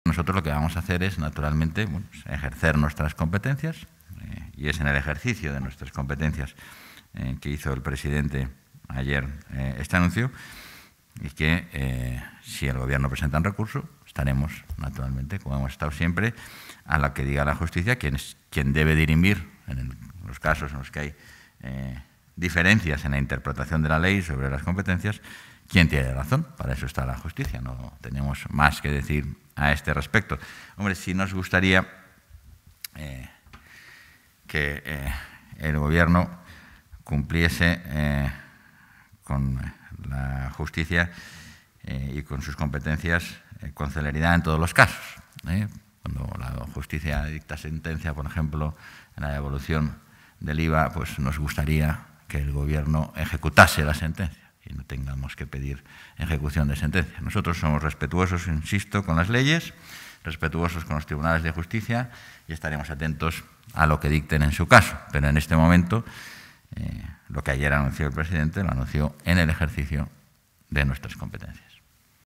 Igea afirma que respetará lo que diga la Justicia sobre Navacerrada
El vicepresidente y portavoz de la Junta de Castilla y León, Francisco Igea, ha afirmado que el Gobierno autonómico respetará lo que diga la Justicia sobre la autorización para que Navacerrada siga abierta y ha pedido al Gobierno central que lo haga también y cumpla con "celeridad" en todos los casos, también la sentencia del IVA.